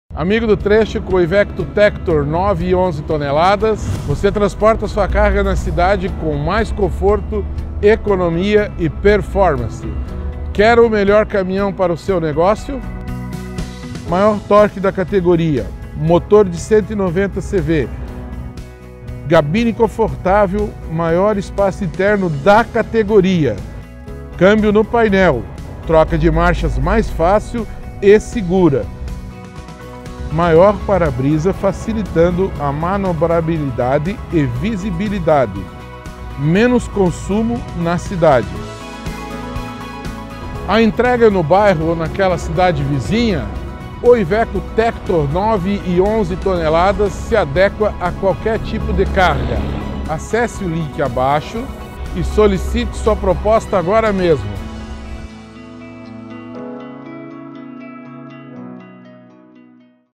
Conforto e versatilidade: Conheça o Tector 9 e 11 Toneladas!
Ampla cabine projetada visando a qualidade de vida a bordo. Ajustes reguláveis, compartimentos inteligentes para não ficar nada para trás. 🚛😍

O IVECO Tector tem a melhor cabine da categoria: para você, um espação.
Mais segurança e facilidade na hora de manobrar, estacionar e enfrentar os desafios do trânsito nas cidades. 👏💪